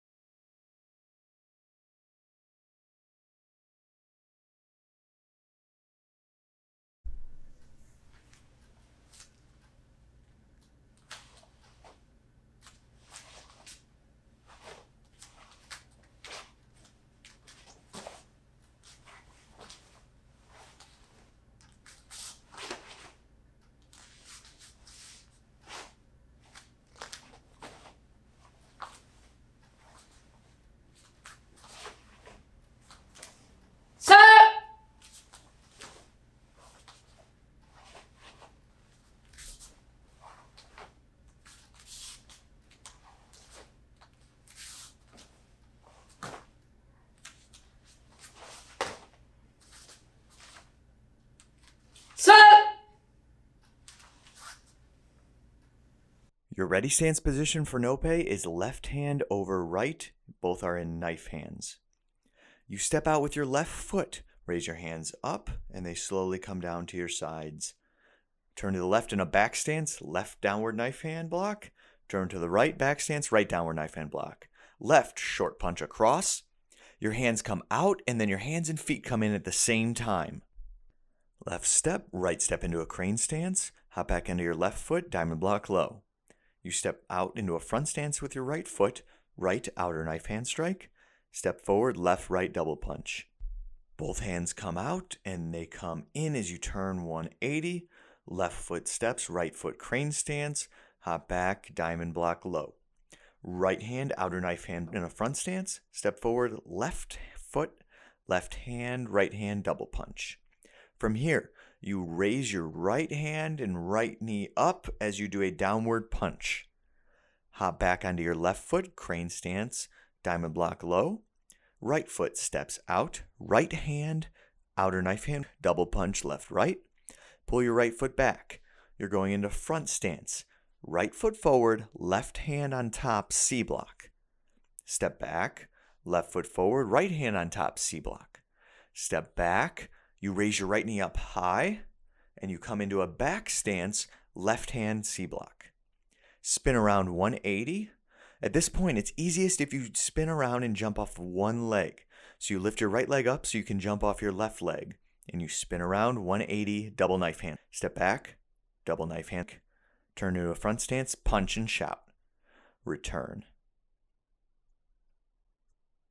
No Pae (with narration)
We demonstrate it twice. The first at a steady pace. The second time is done slower with narration.